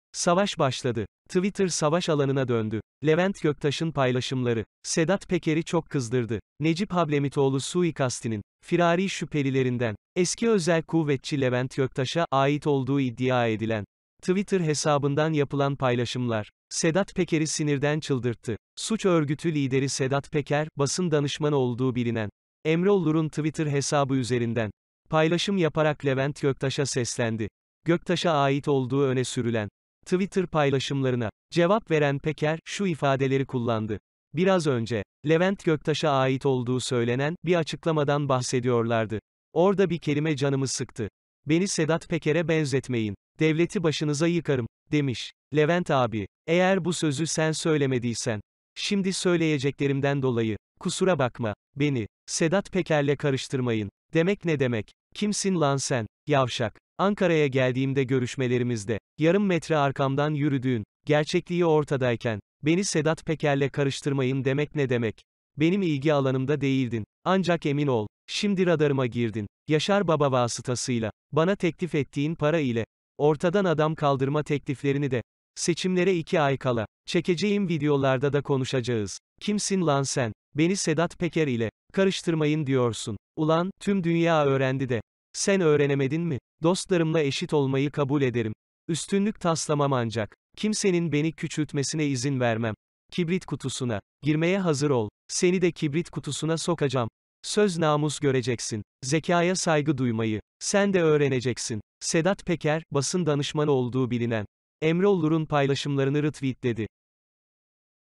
PEKER GECE PATLADI..''KİMSİN LAN YAV.ŞAK''..''SENİ DE KİBRİT KUTUSUNA SOKUCAM''.. SAVAŞ BAŞLADI
Savaş başladı! Twitter savaş alanına döndü! Levent Göktaş’ın paylaşımları! Sedat Peker’i çok kızdırdı!
Necip Hablemitoğlu suikastinin! firari şüphelilerinden! eski özel kuvvetçi Levent Göktaş‘a! ait olduğu iddia edilen! Twitter hesabından yapılan paylaşımlar! Sedat Peker'i sinirden çıldırttı.!
Suç örgütü lideri Sedat Peker! basın danışmanı olduğu bilinen! Emre Olur’un Twitter hesabı üzerinden!  paylaşım yaparak Levent Göktaş’a seslendi.!